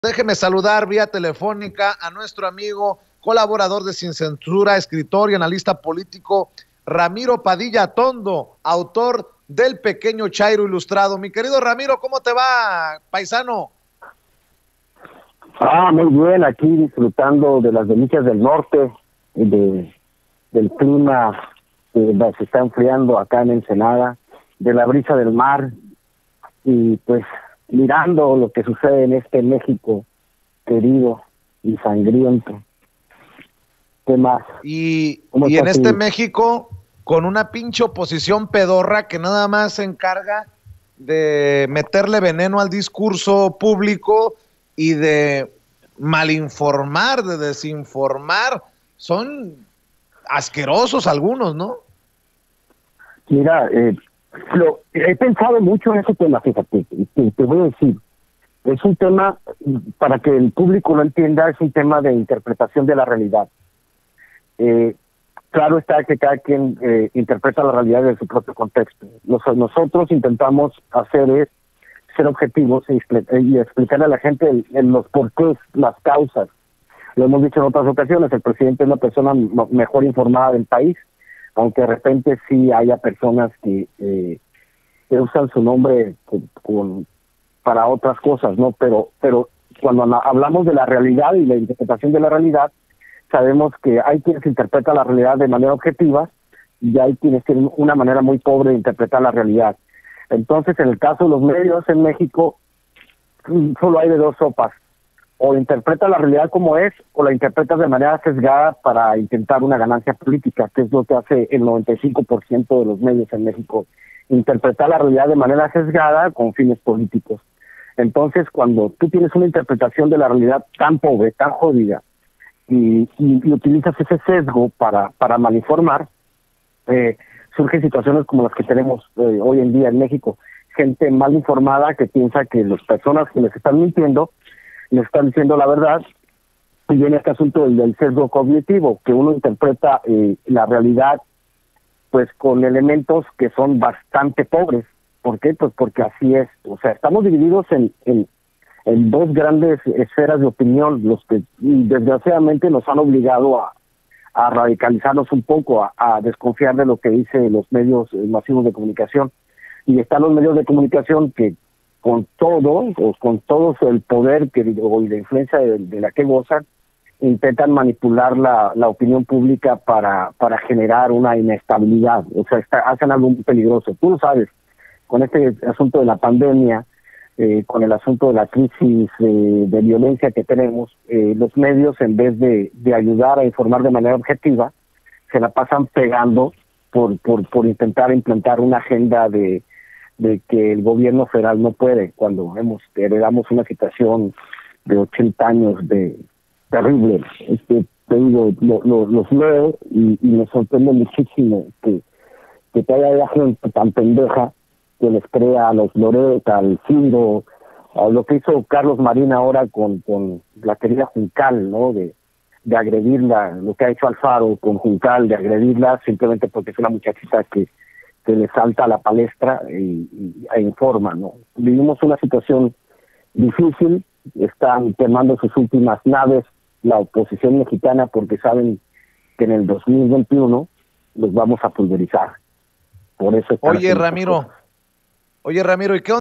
PADILLAATONDO: MEDIOS INTENTAN MANIPULAR LA OPINIÓN PÚBLICA… Y CON LA PANDEMIA ES PELIGROSO
El escritor analistapolítico RamiroPadillaAtondo considera que el presidente López Obrador es la persona mejor informada del país a pesar de que algunos usan su nombre de forma indebida… Nos comentó que con los medios masivos de comunicación solo hay de dos sopas, o interpretan la realidad como es o de manera sesgada por una ganancia y por intereses políticos